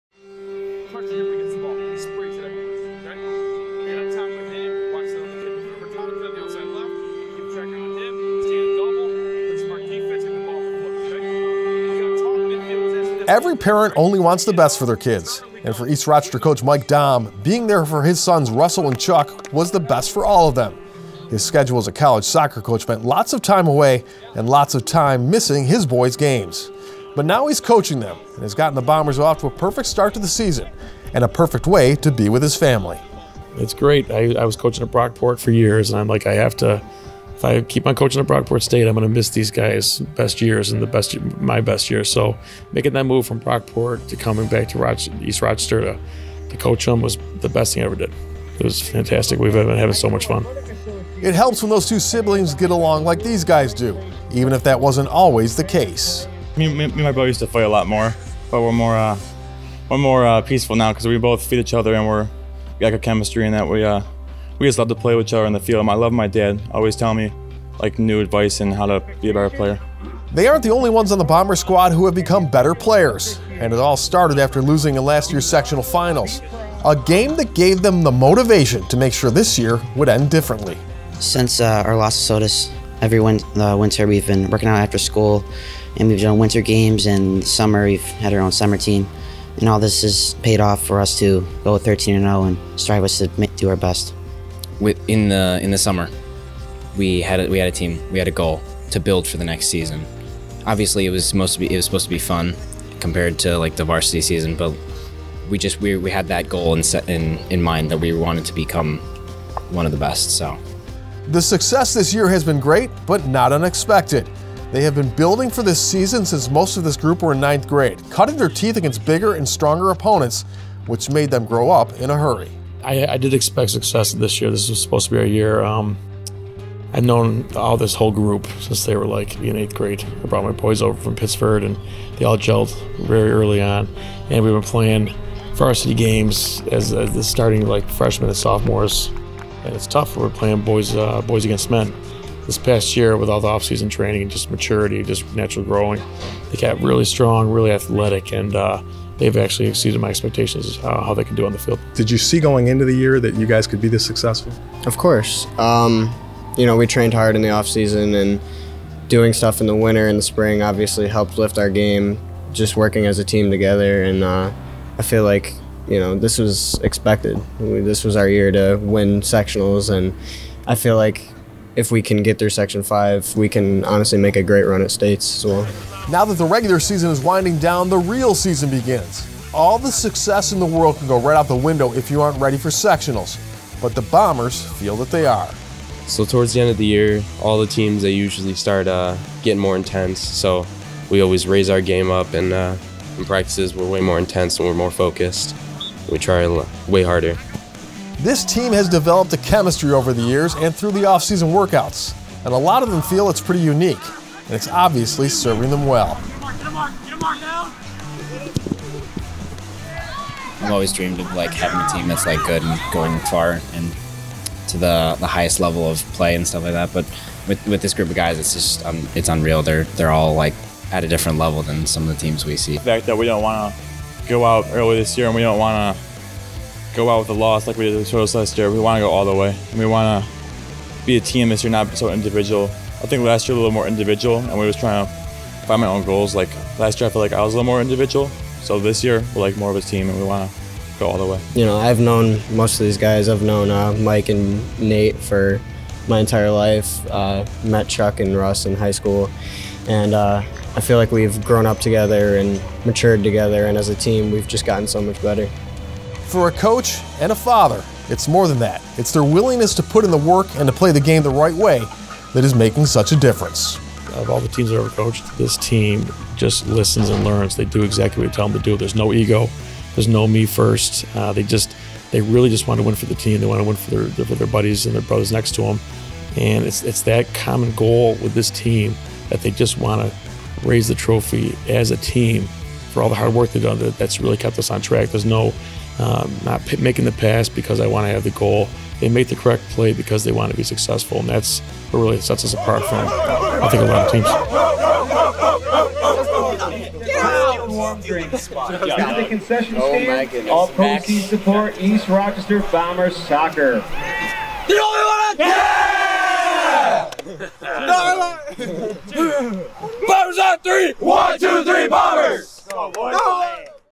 HSSB - East Rochester Boys Soccer - Oct. 19, 2019
EAST ROCHESTER, NY:  The East Rochester boys soccer team has had a perfect season so far this year, and it’s not a surprise to head Coach Mike Domm - the former Rhino knew that this could be a big year for the Bombers...